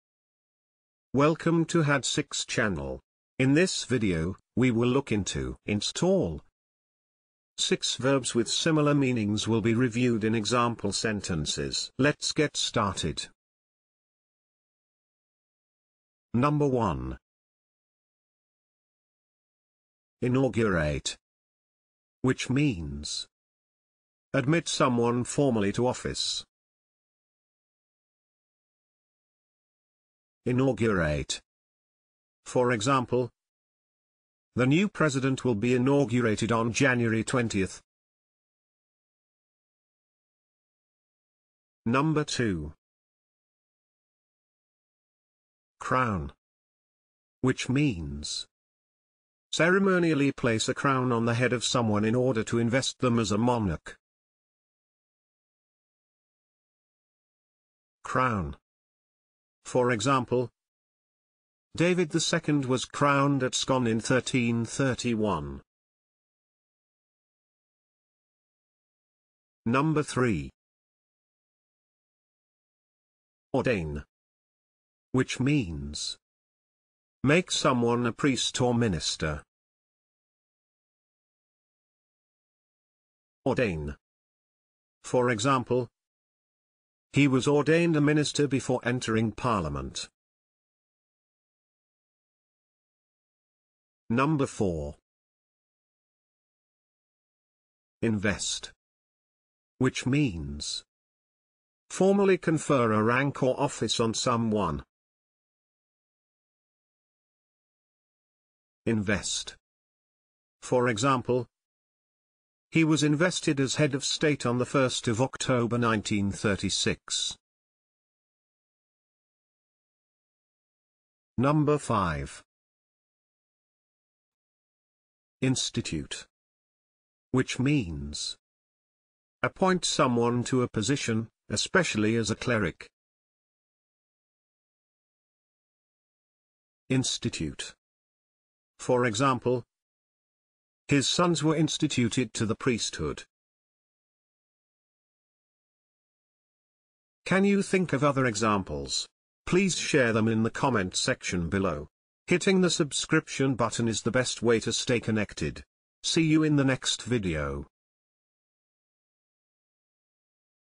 install - 6 verbs synonym to install (sentence examples)
This video examines install as a verb. Here, 6 verbs, which have meanings similar to that of install in standard English, are introduced through sentence examples. These synonyms help you to remember the meanings of this group of words for improving your vocabulary.